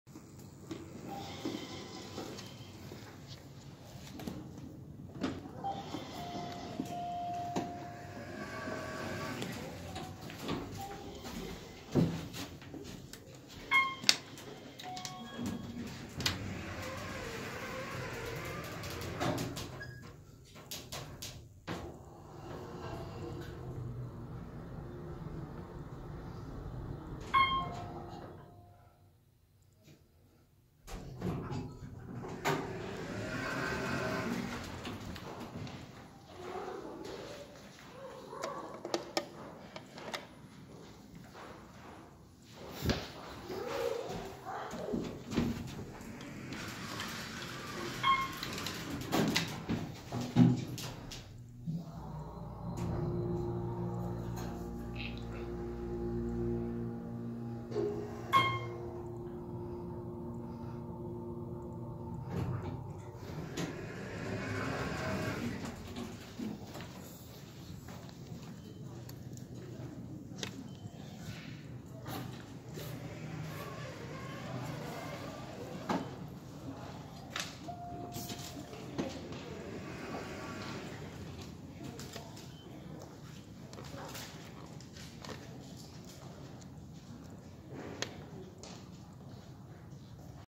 Another Strange Otis Series 1 Elevator at Mount Prospect Public Library, Mount Prospect, IL
This is Car B. It takes you to the parking garage. The elevator is the same as the previous one. Furthermore, they were both installed in 2003.